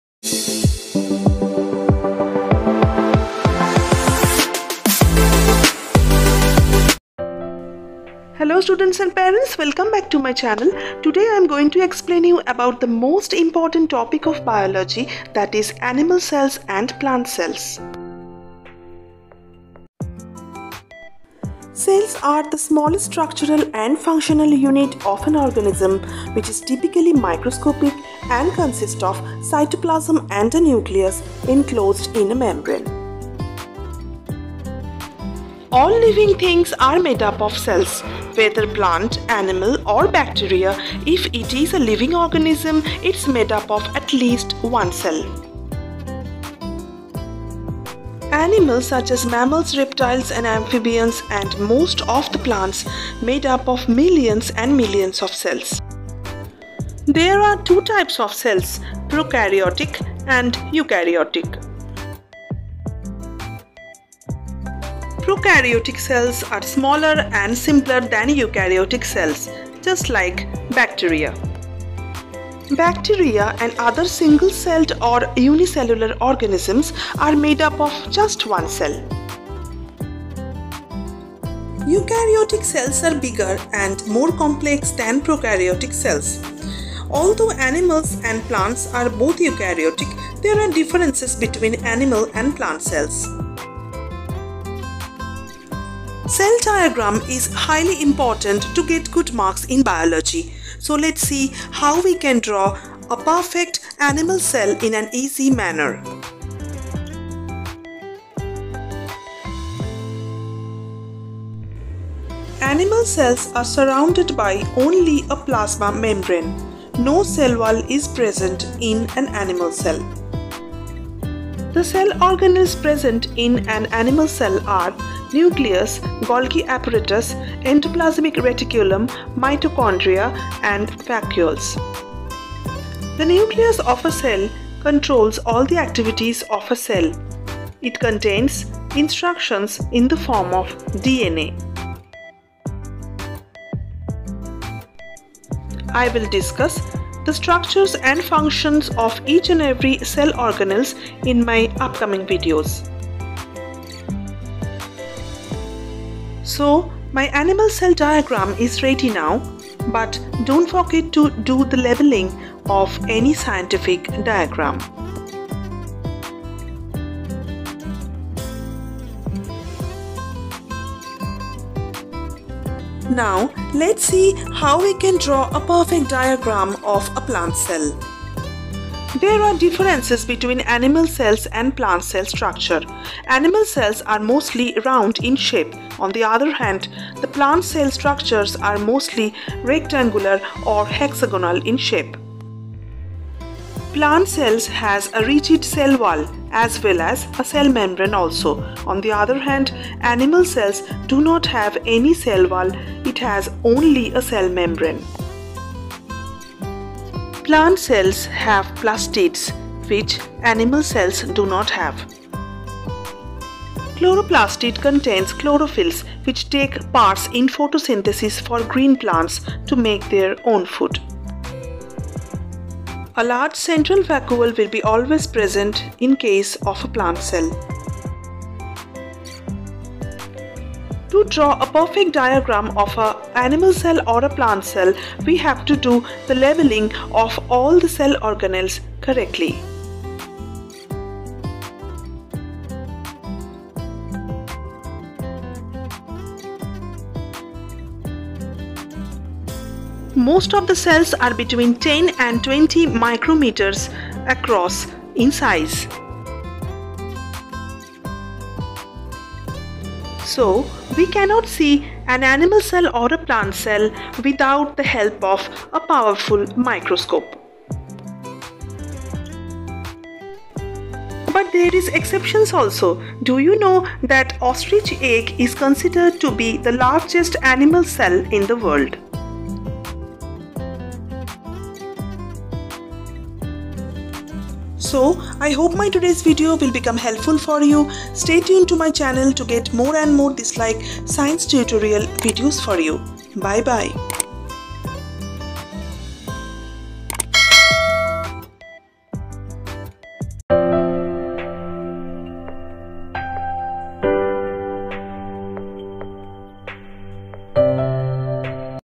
Animal Cells and Plant Cells | Science Tutorial For School Students
Hello viewers, in my today's video I explained the structure and function of animal cells and plant cells.I also described how to draw the diagram of an animal cell and a plant cell in an easy manner. So watch my video till the last end without dragging and be the first viewer to get more and more this like amazing videos for you.